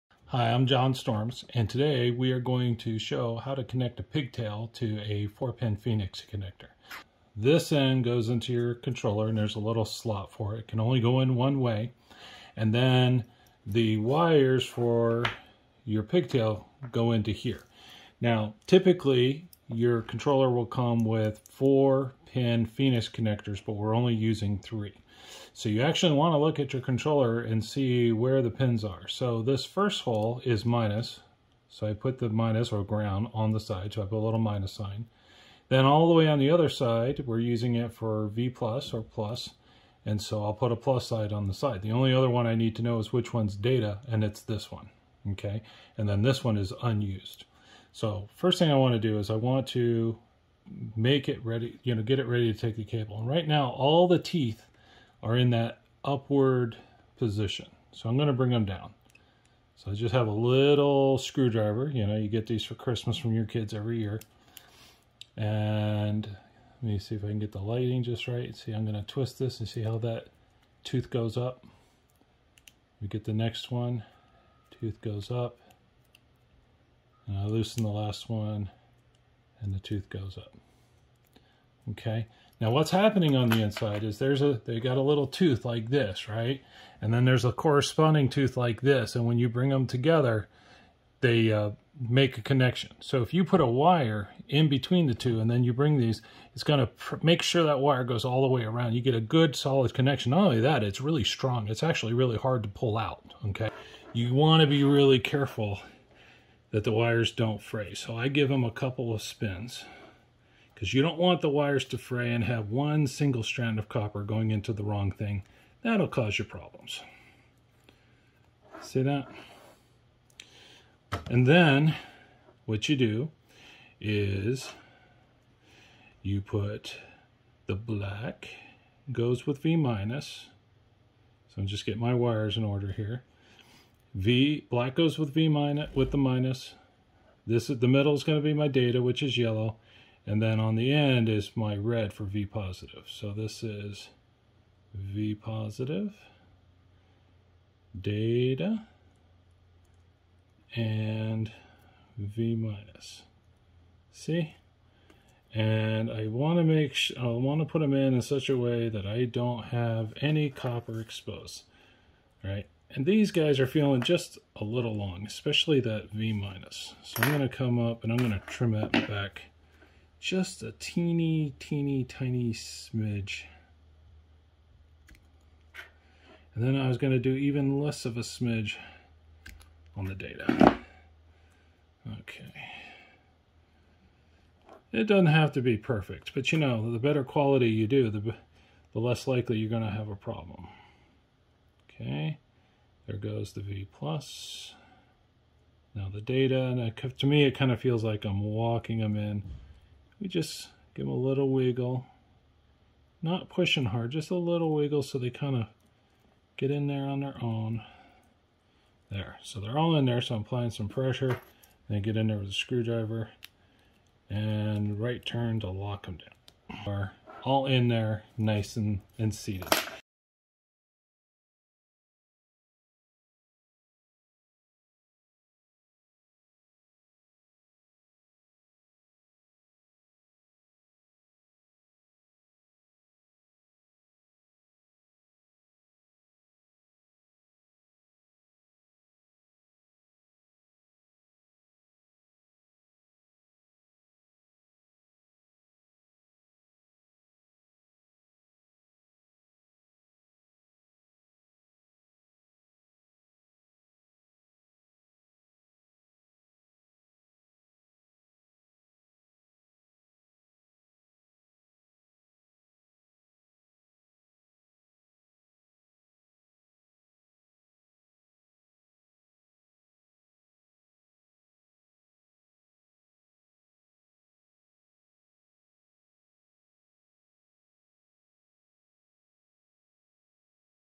Falcon Controller Build Part-7: Connecting Pigtails
Seventh video in a series on how-to do a Falcon controller build with a Bud Box enclosure. When complete the controller will consist of a Falcon F16v3 plus 2 extensions providing 48 pixel ports. In this video I connect the 48 pigtails to phoenix connectors then plug them into the controller boards.

Tools:
Small Flat head screwdriver